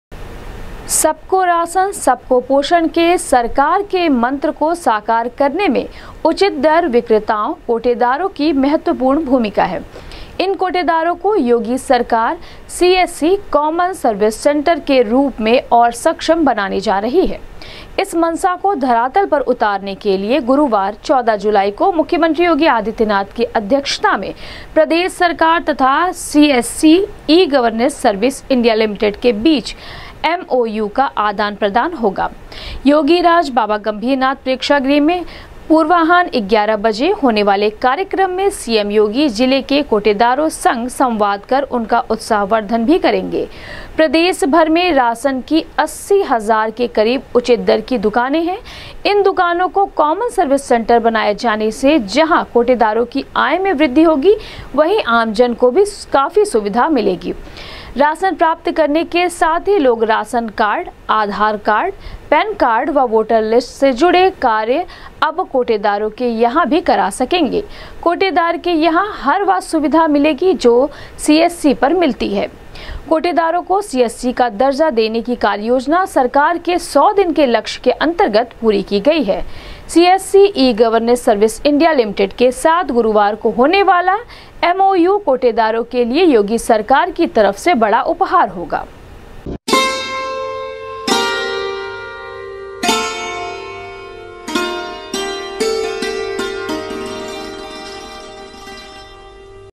गुरुवार को कोटेदारों को सीएससी के रूप में सक्षम बनाएगी योगी सरकार, मुख्यमंत्री की मौजूदगी में प्रदेश
गुरुवार को कोटेदारों को सीएससी के रूप में सक्षम बनाएगी योगी सरकार, मुख्यमंत्री की मौजूदगी में प्रदेश सरकार तथा सीएससी ई-गवर्नेंस सर्विस इंडिया लिमिटेड के बीच हस्ताक्षरित होगा एमओयू, योगीराज बाबा गंभीरनाथ प्रेक्षागृह में कोटेदारों संग संवाद करेंगे सीएम योगी